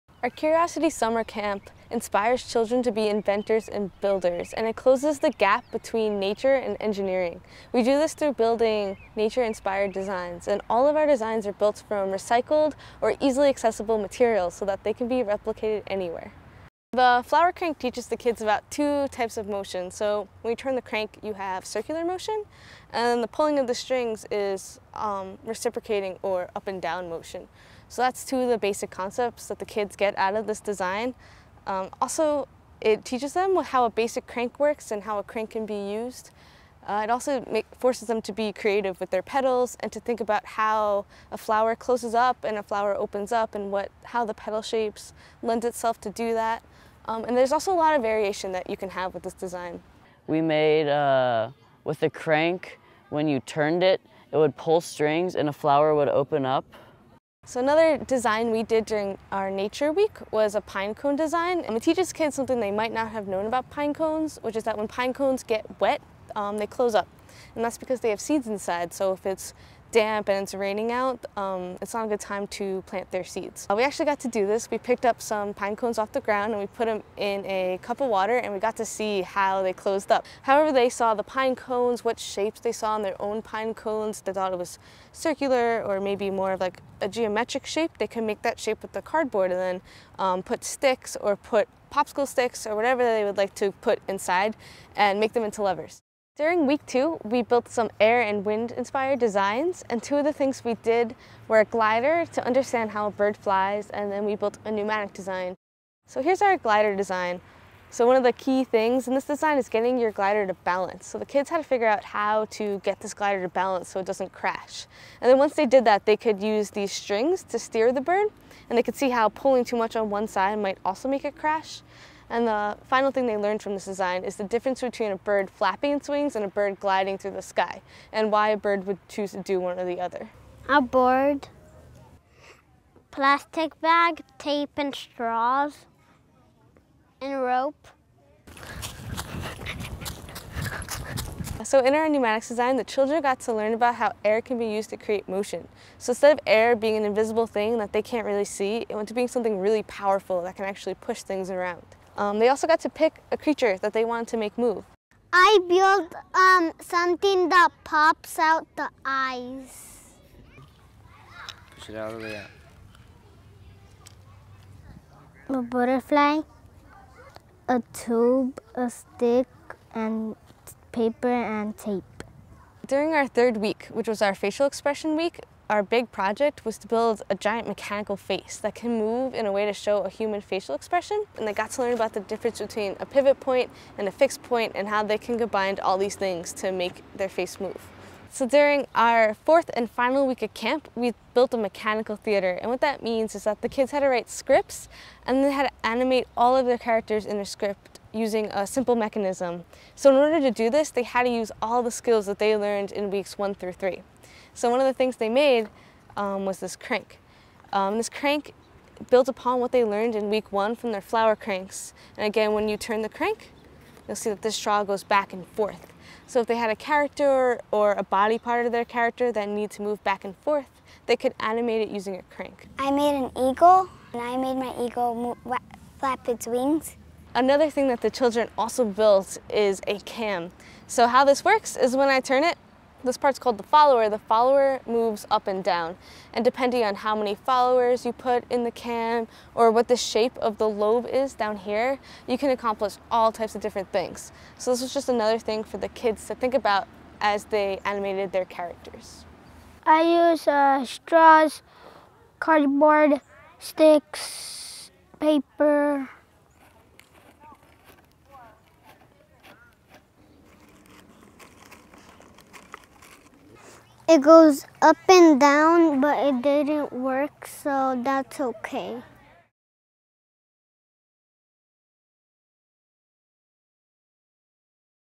Engineering in Nature with Kindergarteners
Flower Cranks, Pine Cones, Gliding Birds, Facial Expressions...
Little engineers paired with high school students for 4 inspiring summer weeks and built together nature inspired designs, in the middle of a redwood forest in California. Watch here the results of the Curiosity Summer Camp and get inspired to build the same designs with the support of curiositymachine.org

Director: Cris Meira
Edit: Cris Meira
Script: Mariana Rutigliano and Alli Coyler
Illustrations: Audra Torres